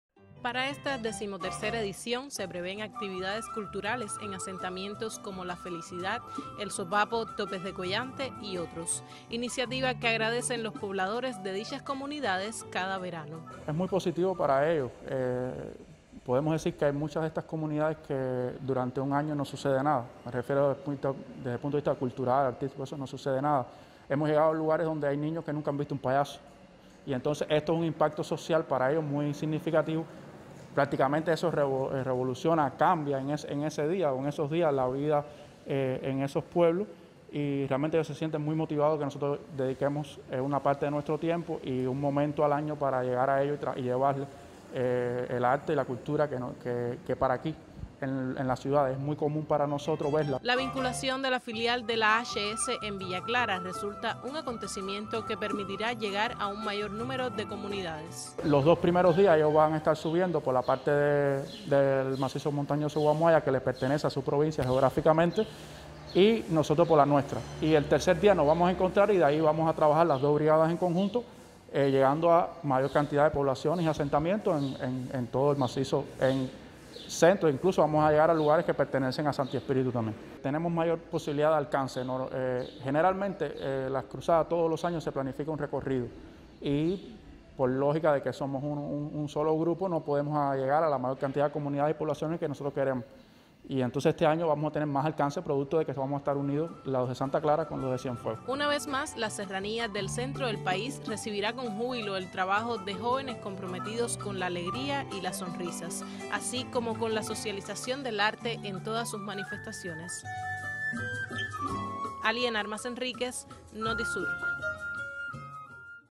Prepara AHS Cienfuegos cruzada por la zona montañosa.
Del 3 y al 9 de agosto tendrá lugar la cruzada artístico-literaria evento que cada año organiza la Asociación Hermanos Saíz, para llegar con su arte a las comunidades más apartadas en el macizo montañoso Guamuhaya.